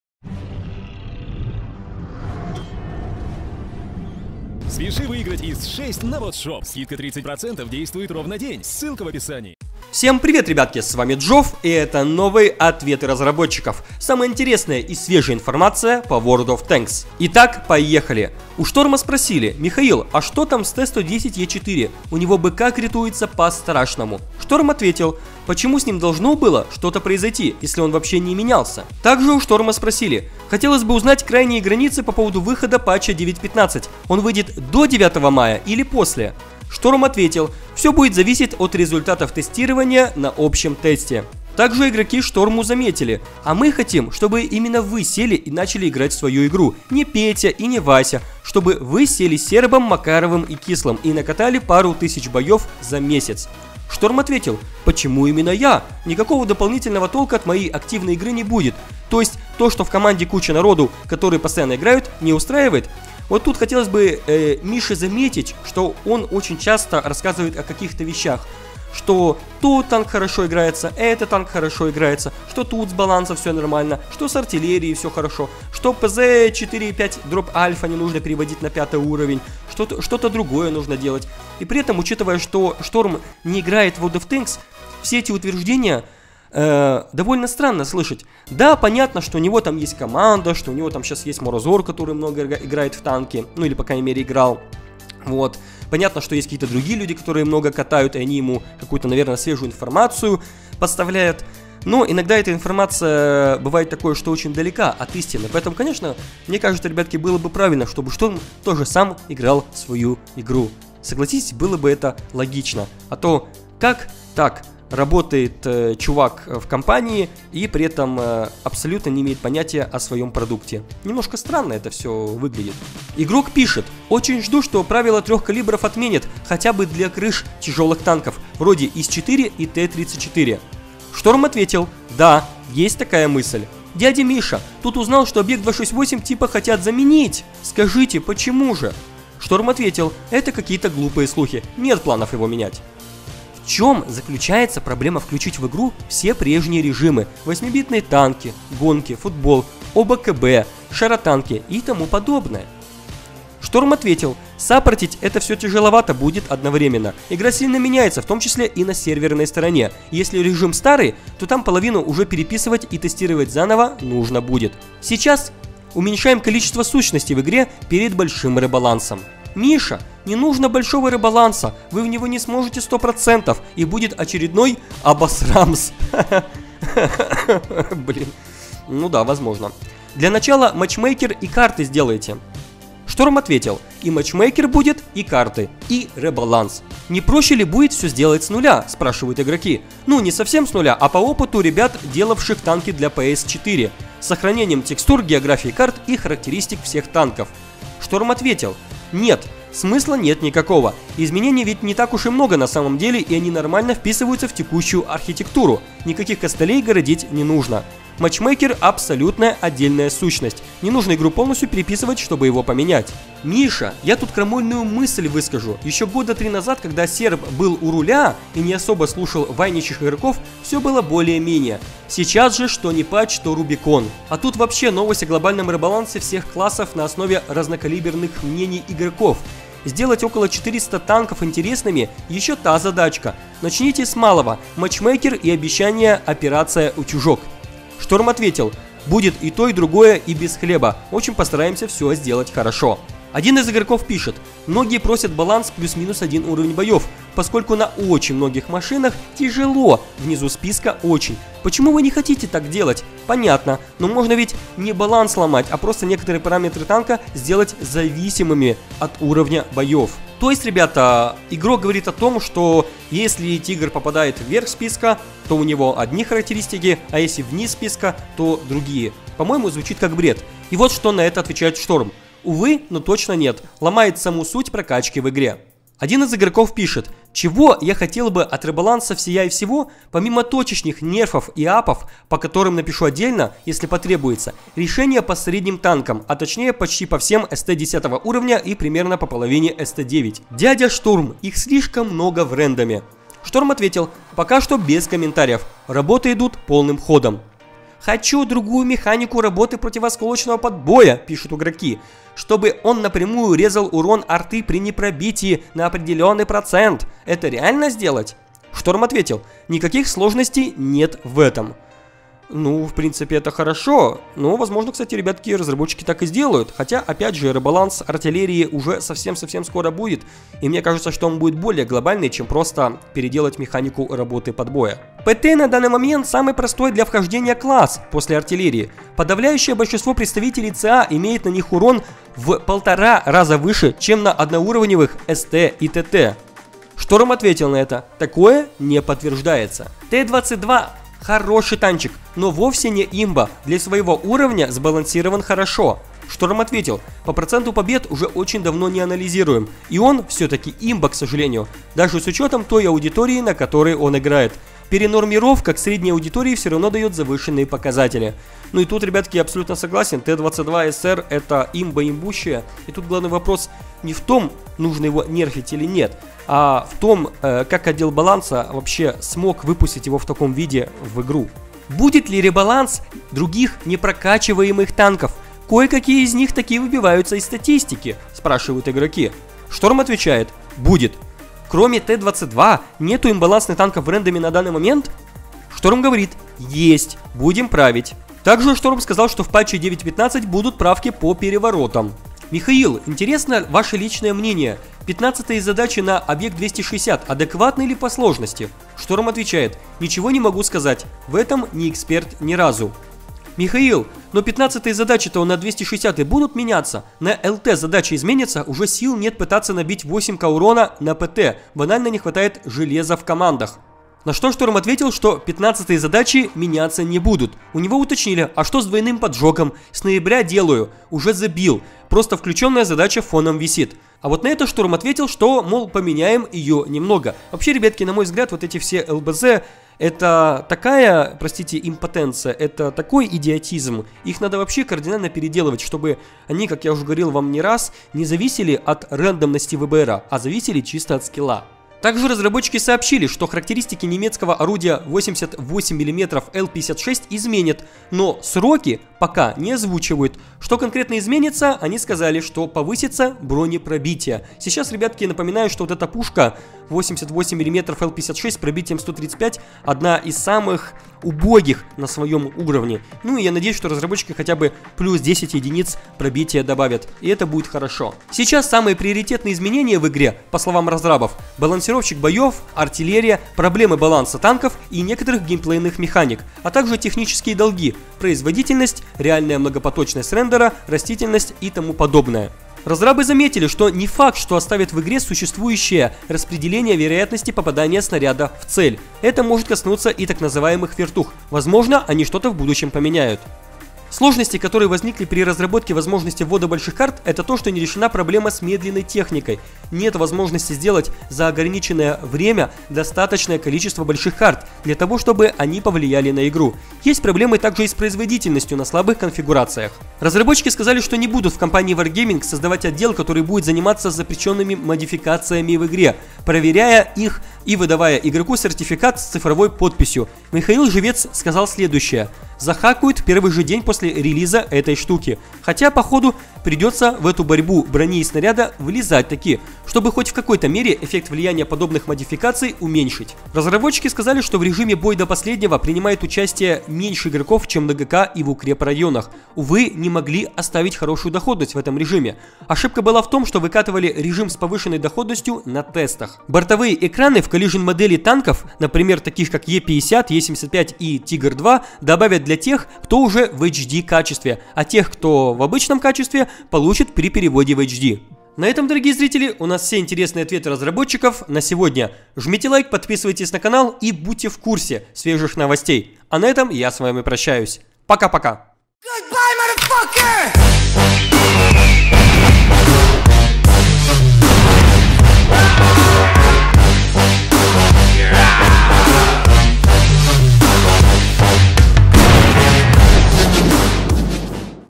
Ответы Разработчиков #109. Ап 8.8 L/56, правило трёх калибров для ТТ и ребаланс ЛБЗ.
Свежие новости по WoT на канале!

Разработчики снова думают над изменением точности танков и возможно пересмотрят правило трех калибров для некоторых тяжелых танков. Также в 9.15 апнут многострадальную немецкую пушку 8.8 L/56. А еще переделают идиотские ЛБЗ, связанные с поджогами нескольких танков.